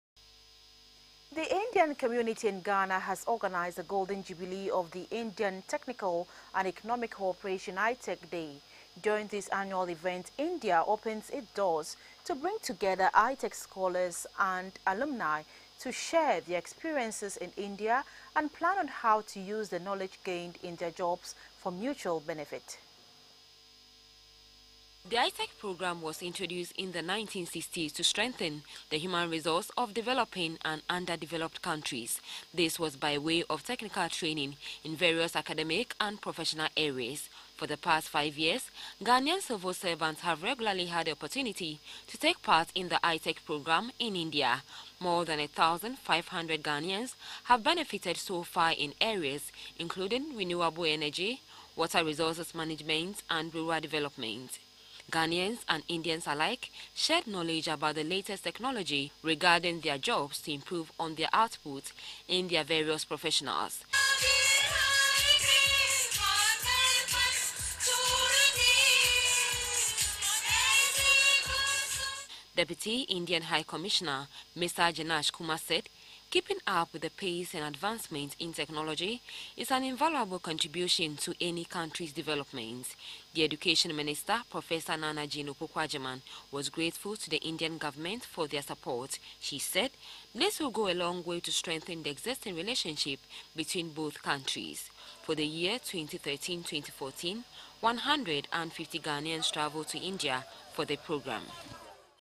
Golden Jubilee of the Indian Technical and Economic Cooperation in Ghana
The Indian community in Ghana has organised the golden jubilee of the Indian Technical and Economic Cooperation (ITEC) day.
During this annual event, India opens its doors to bring together ITEC scholars and alumni to share their experiences in India and plan on how to use the knowledge gained in their jobs for mutual benefit.